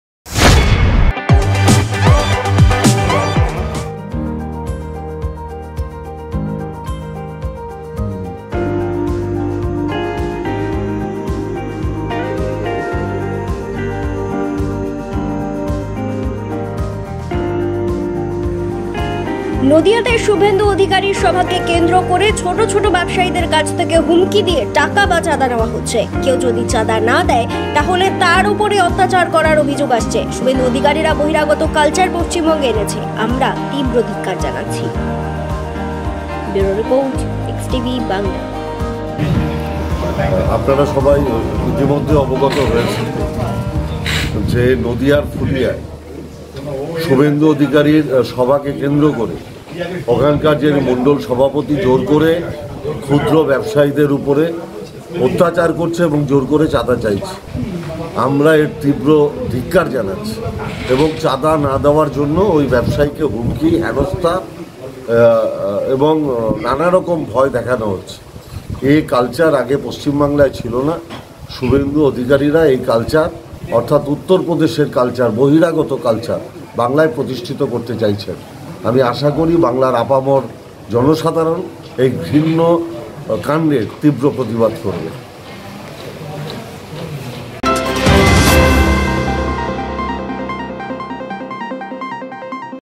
শুভেন্দু অধিকারী সভাকে কেন্দ্র করে ছোট ব্যবসায়ীদের কাছ থেকে হুমকি দিয়ে টাকা নেওয়া হচ্ছে।
নদীয়াতে শুভেন্দু অধিকারী সভাকে কেন্দ্র করে ছোট ছোট ব্যবসায়ীদের কাছ থেকে হুমকি দিয়ে টাকা বা চাঁদা নেওয়া হচ্ছে।